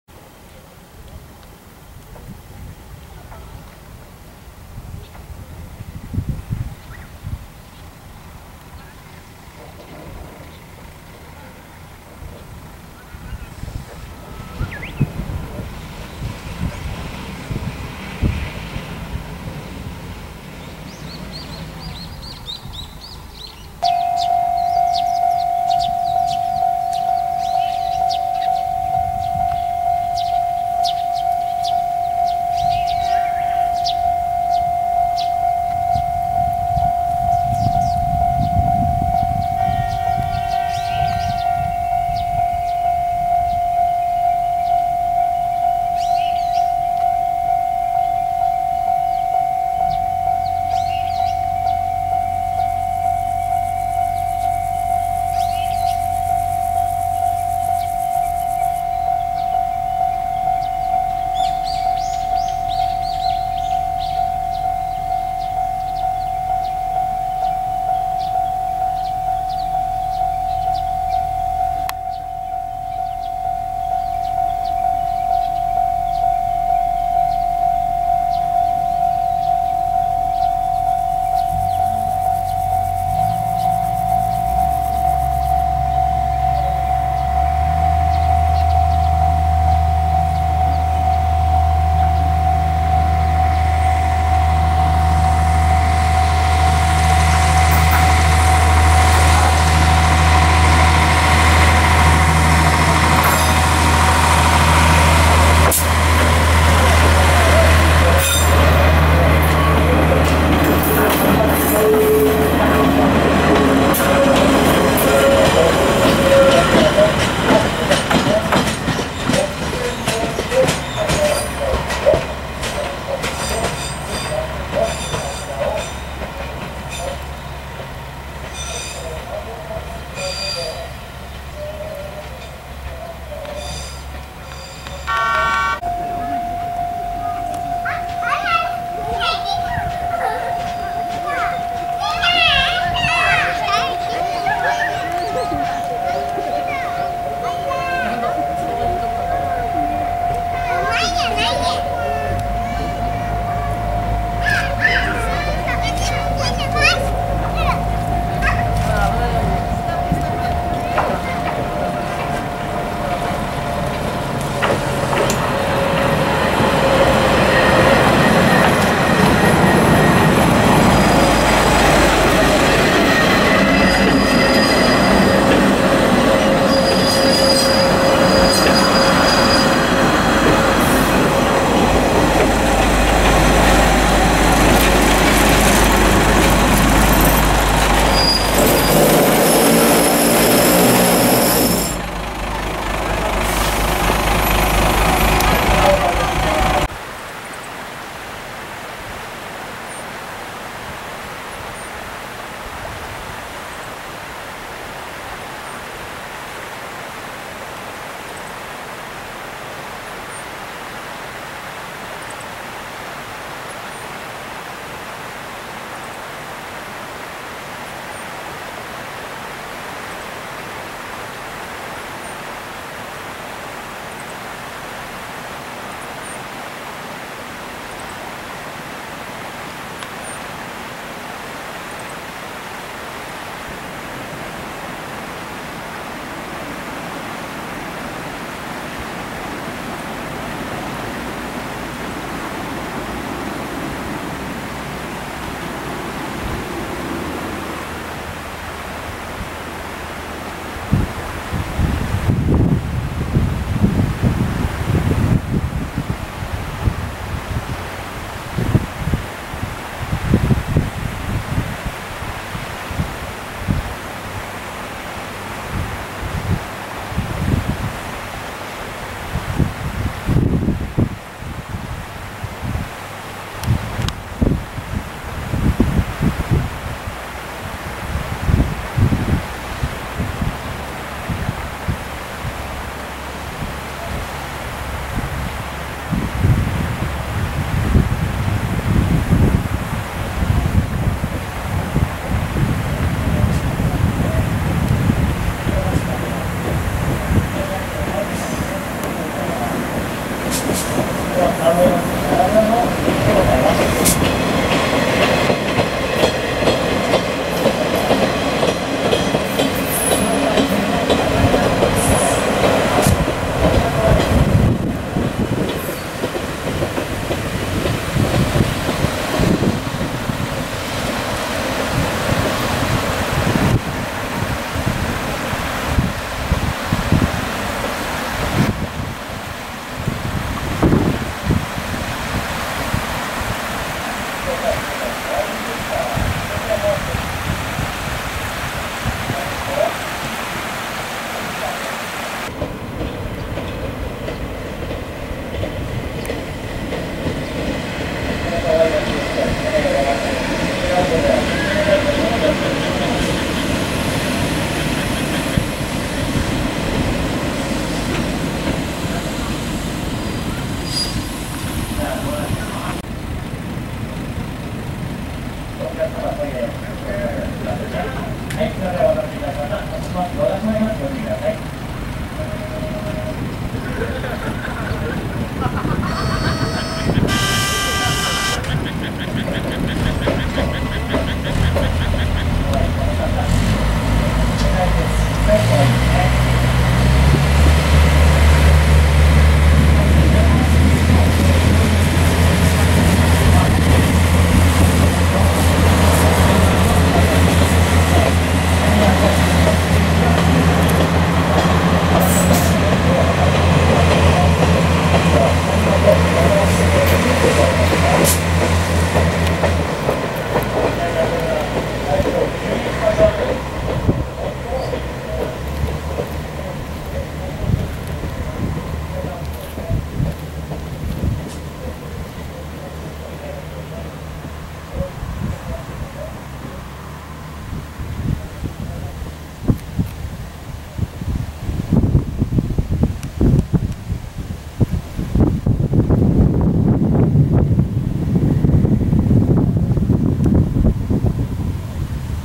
南阿蘇鉄道のトロッコ列車を沿線などから撮影
南阿蘇鉄道の大人気のトロッコ列車を、沿線などから何度か撮影しました。高森駅に入線するところ、発車するところ、南阿蘇白川水源駅、阿蘇白川駅のそれぞれの周辺で撮っています。実際に乗車もしましたが、実に良いですね。沿線ののどかな雰囲気も素晴らしい。4年後の、全面再開の折に、再訪したいと思います。